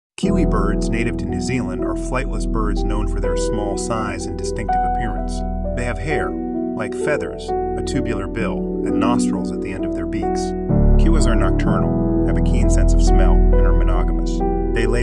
kiwi bird an endangered bird
The kiwi bird is a unique and iconic bird native to New Zealand